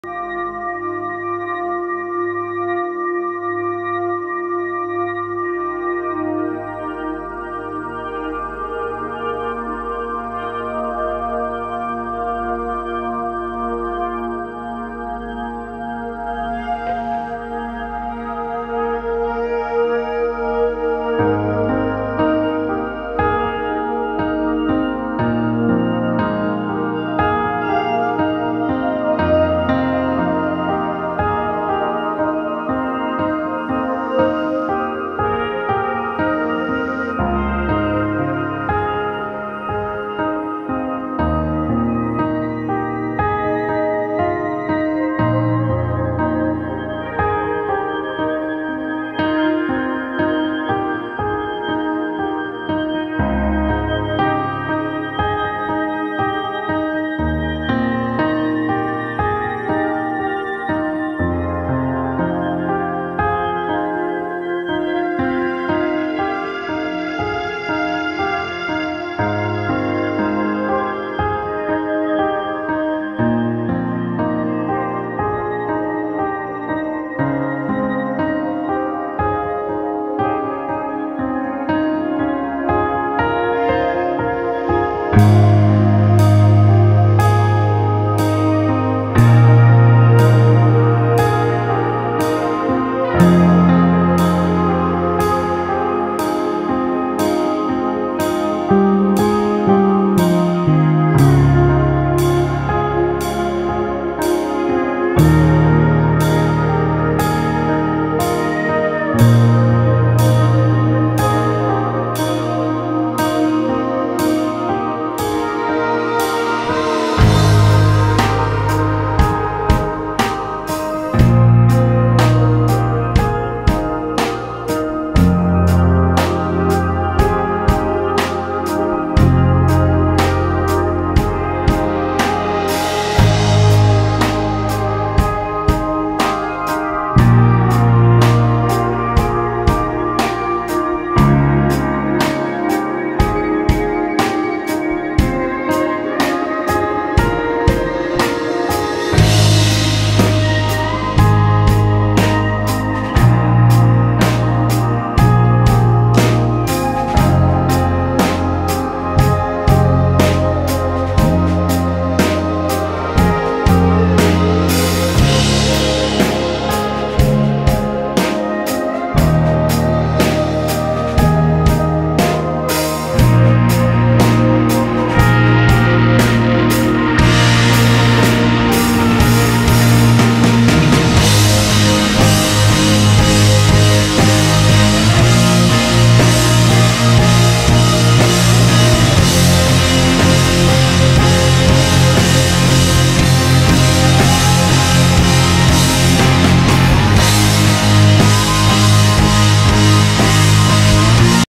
Operation Air Bridge - LAPES
Brazillian Air Force C130H performing LAPES.
WARR-WADE-WARR